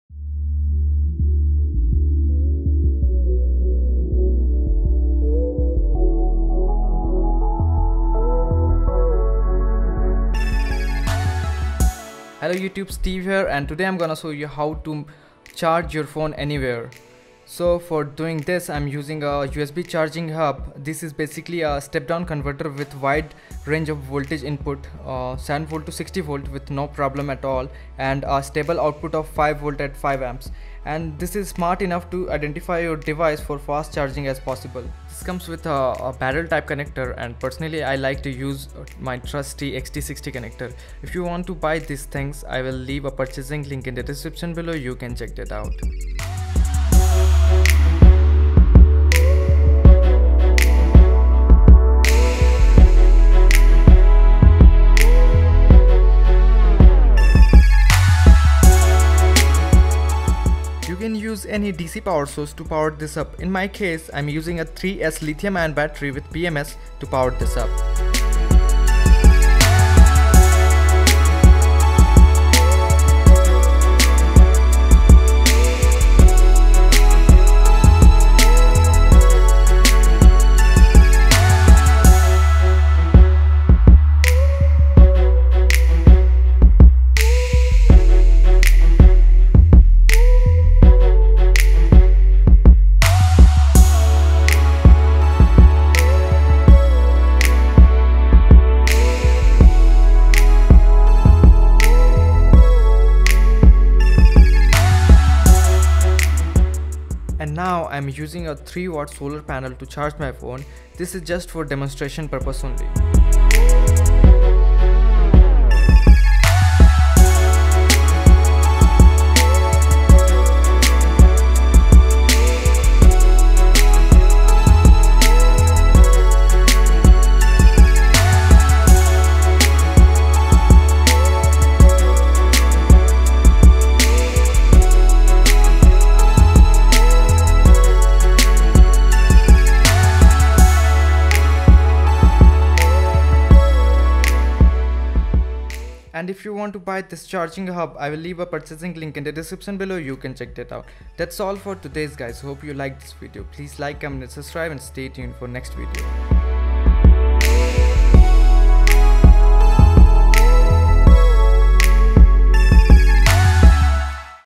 Charge Your Phone Anywhere
Hi YouTube in This video I'm Going to Show You How Charge Your Phone Anywhere

Banggood

Aliexpress

1. USB Charging HUB -

Amazon

1. USB Charging HUB -
2. 3S BMS
3. 4S BMS
4. 18650 Battery
5. XT60 Connectors
6. Solar Panel -

Prime Lense
Main Lense
Editing Mechine